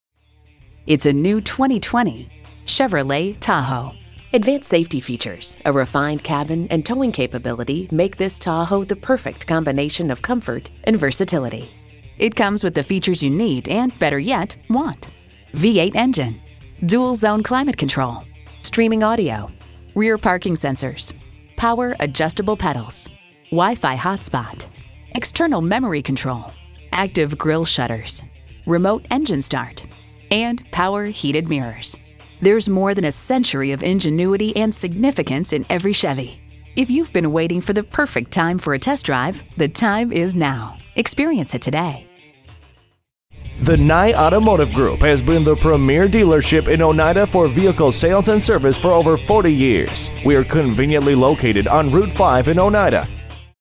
2020 Chevrolet Tahoe Utica NY Oneida, NY EV0582 - SOLD
Year: 2020
Make: Chevrolet
Model: Tahoe
Trim: LS
Engine: EcoTec3 5.3L V8
Transmission: 6-Speed Automatic Electronic with Overdrive
Color: Black
Interior: Jet Black
Stock #: EV0582
VIN: 1GNSKAKC1LR191161
Black 2020 Chevrolet Tahoe LS 4WD 6-Speed Automatic Electronic with Overdrive EcoTec3 5.3L V8 4WD, Jet Black.

Address:
1441 Genesee St.